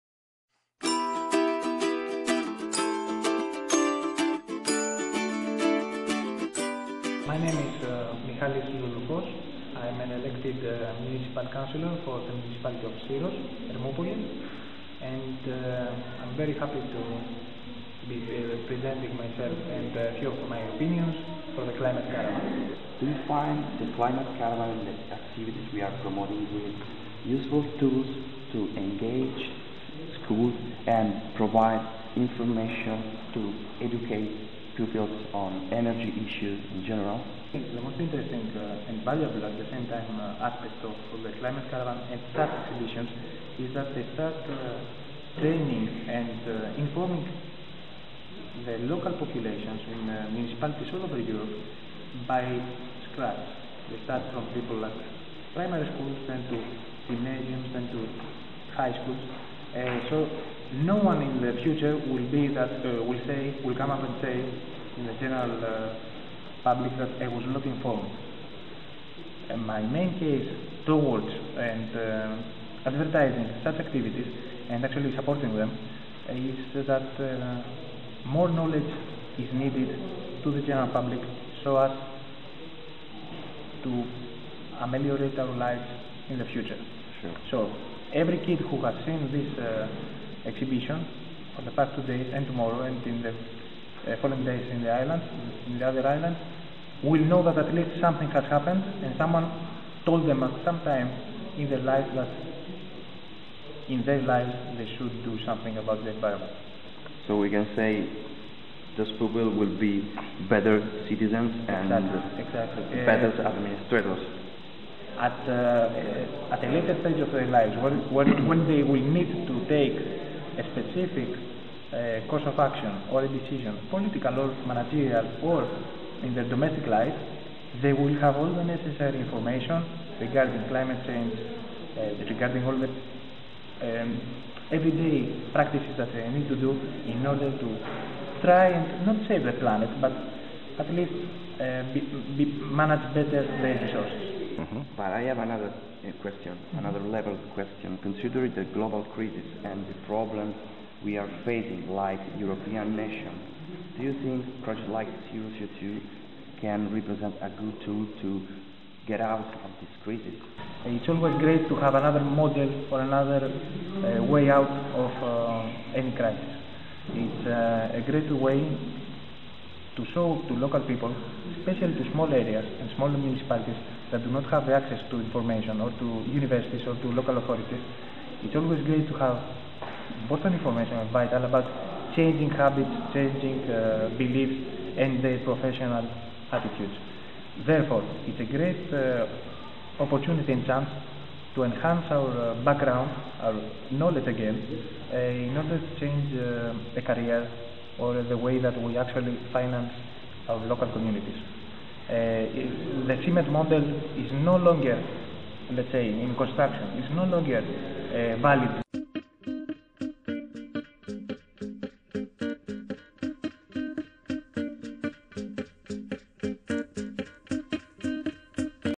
Climate Caravan logbook - diario di bordo Carovana del Clima
La Carovana del Clima alla sua terza tappa
06/10/2011 Grecia, Syros, Isole Cicladi; il diario dei giorni greci della Carovana del Clima, campagna inerante, internazionale del progetto MED ZEroCO2.